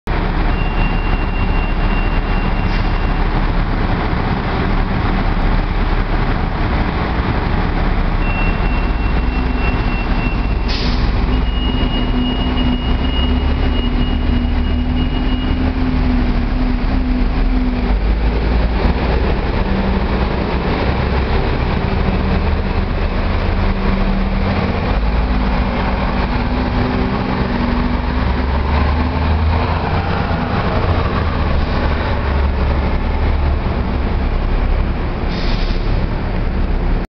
CAMTRAN Gillig Bus
2007 Gillig bus 727 on the route 20 taking its last trip to westmont and brownstown on this nice saturday afternoon